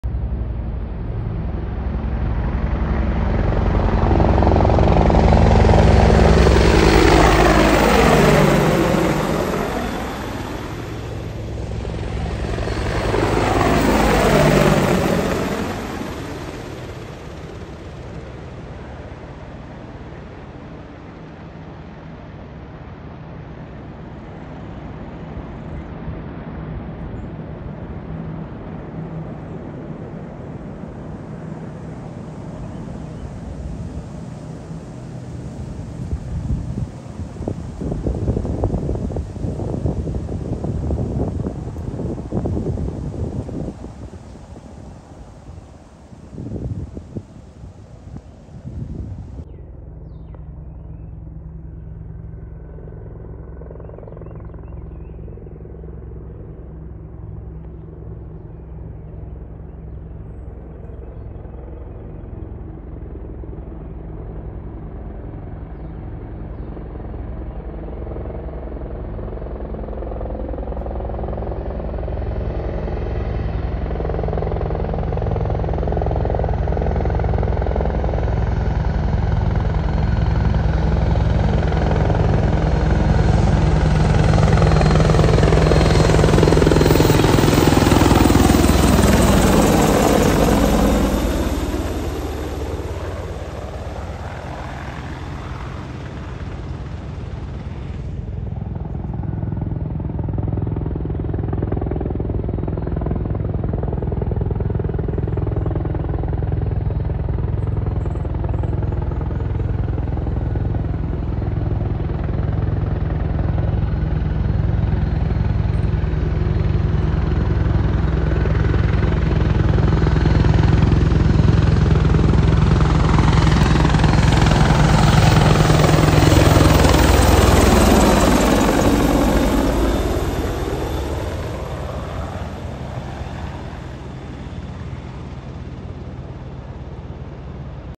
2 NC National Guard Boeing AH-64E Apache Guardian helicopters flying over the Cape Fear River
(06/25/2024)
Beautiful 🌅 evening to see my friends 💙 the pilots passed by the Cape Fear River.🫡🤙
North Carolina National Guard
1-130th Attack Battalion - AB

1st
Flight Info
Airline: United States - US Army
Aircraft: Boeing AH-64E Apache Guardian
Reg: 21-03412
Livery: Normal
Airport: Raleigh-Durham International Airport
Runway: 32
Flight: ZEUS22
Route: RDU-RDU
Date: 06/25/24
Time: N/A

2nd
Flight Info
Reg: 21-03416
Livery: Normal
Runway: 32
Flight: ZEUS21
Route: RDU-RDU
Date: 06/25/24
Time: N/A